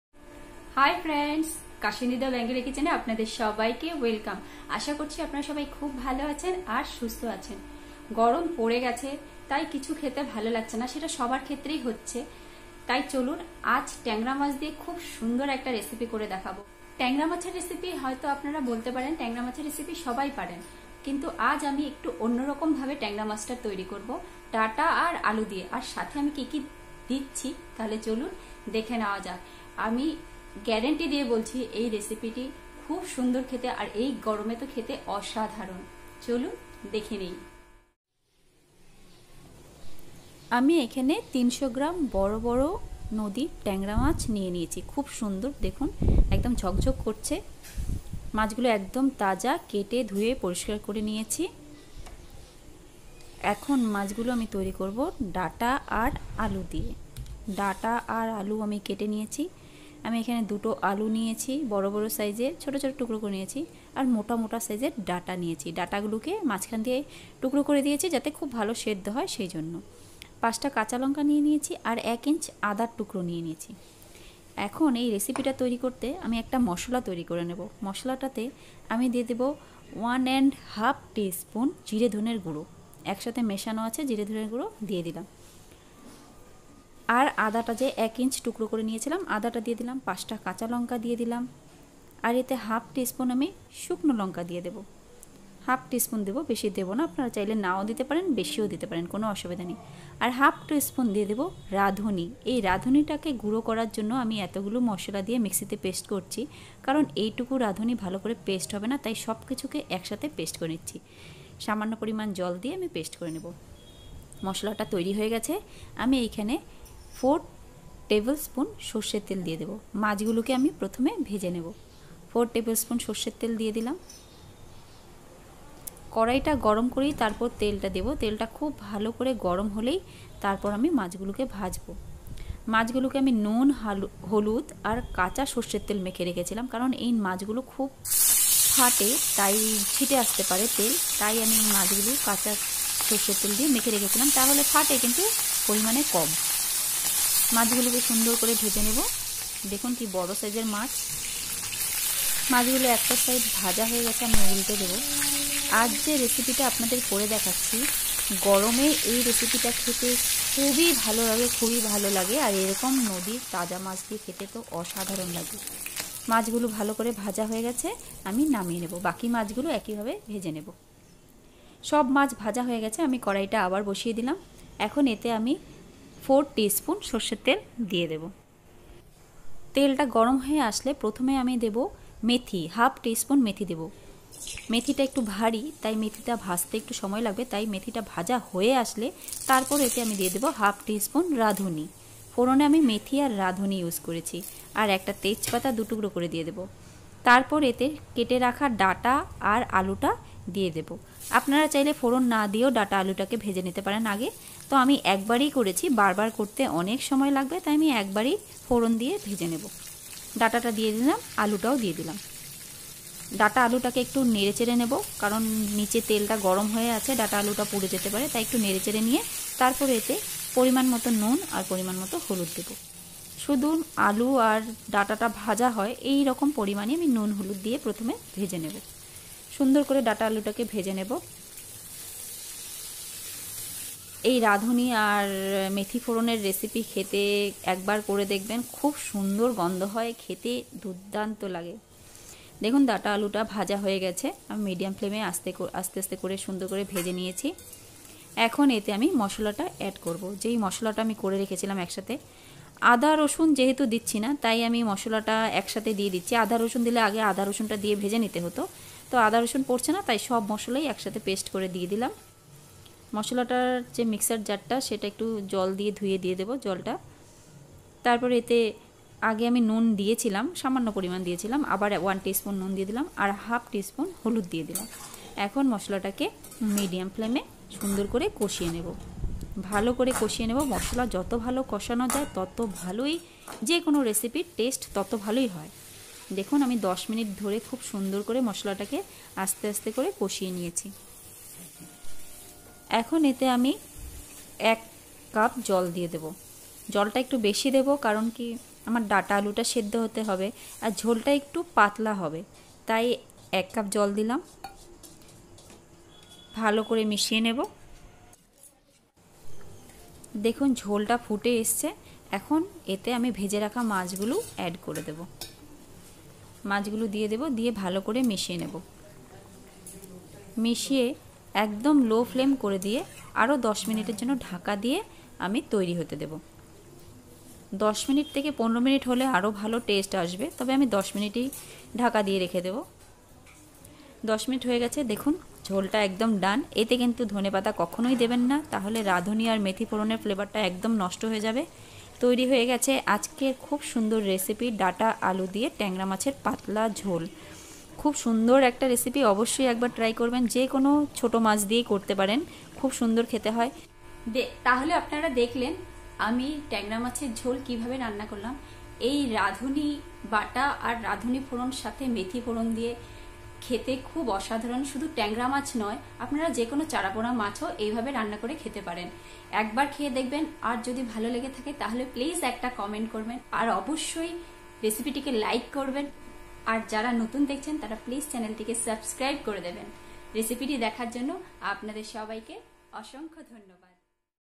Data Aloo r Radhuni Bata Diye Tangra Macher Jhol Ekbar Khele Barbar Khete Chaiben .
Visit my another recipe
Tangra Macher jhal :-